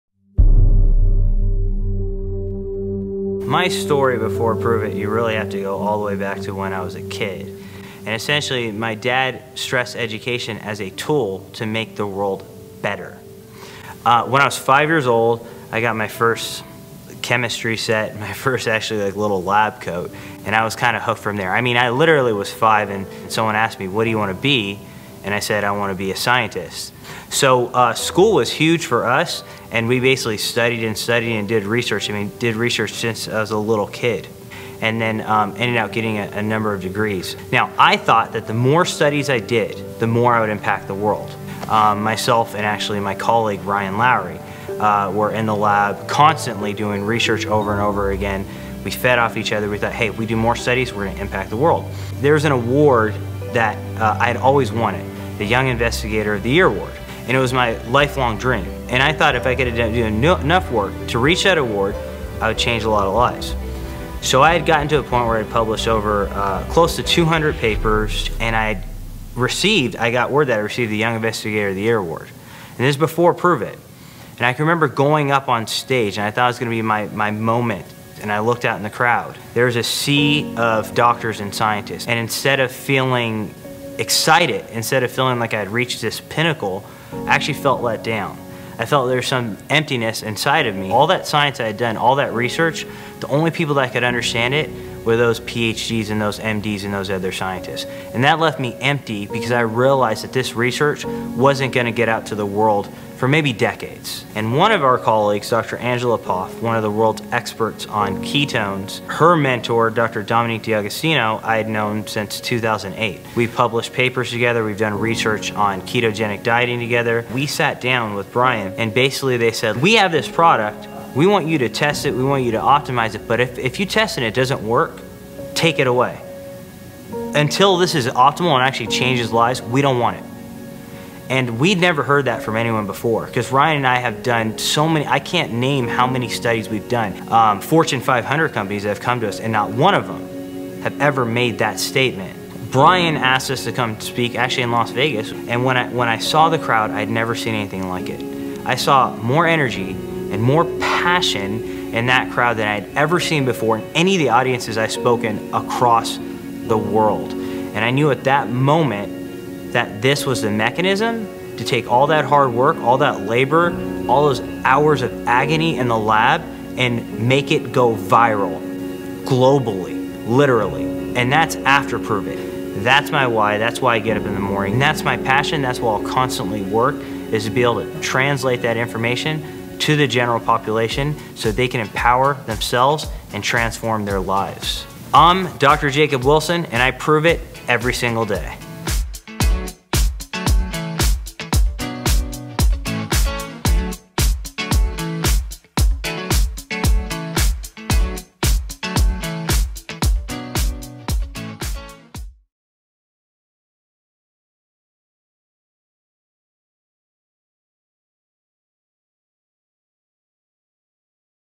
Dr. Jacob Wilson // Inspire ON
"Brian (Underwood) asked us to come speak in Las Vegas and when I saw the crowd, I saw nothing like it. I knew at that moment, this was the mechanism to take all of that hard work, all that labor, hours of agony in the lab, to make it go viral, globally! That's my passion, that's my IT. To be able to take the information and translate it to the general population so they can empower themselves, and transform their lives "

I am Dr. Jacob Wilson, and I Prüvit EVERY. SINGLE. DAY!

Hit PLAY to hear more of Dr. Jacob's story. How his lifelong dream became unfulfilling until he had the platform to actually reach millions of people!